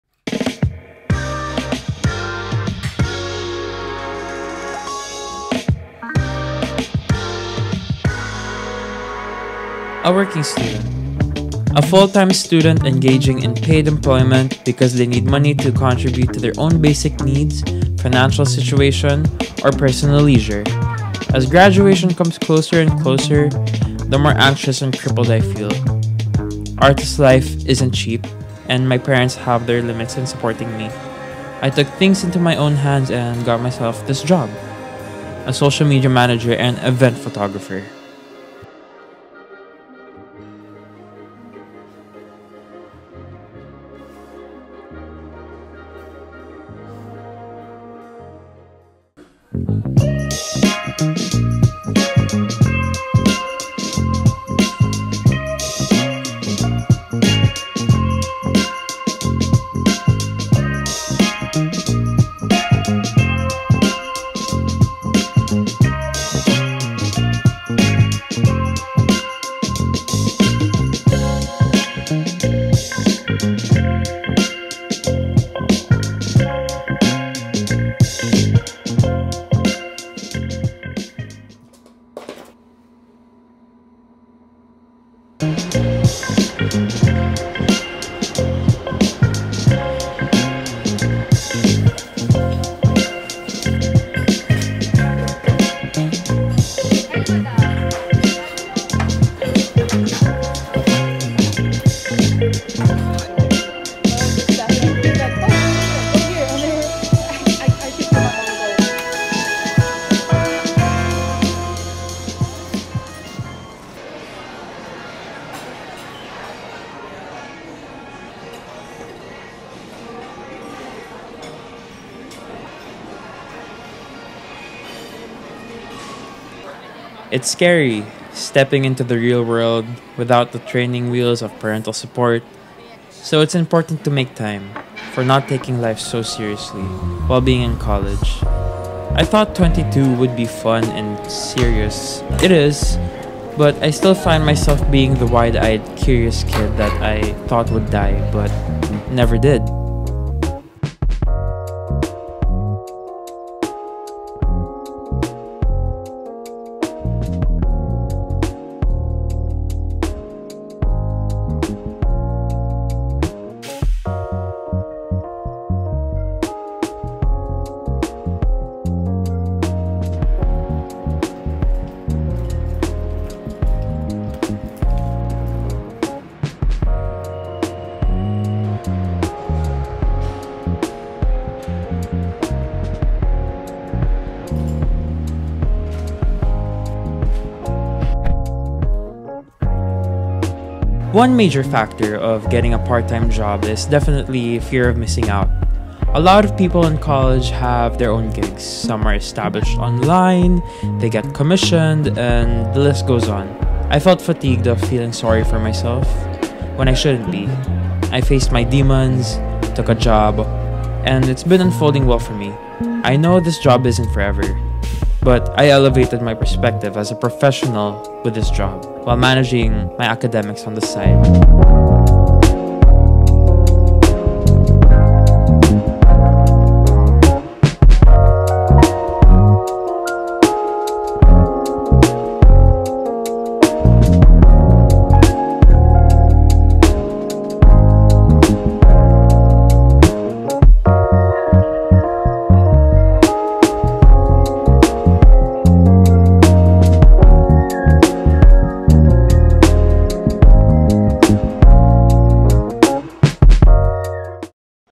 Life Of A Working Student
This is a short video on my life as a working multimedia arts student. I first show one of the few exciting parts of my part-time job. Then give a scope of a fun day with my college friends at the mall, and family friends at a ballet.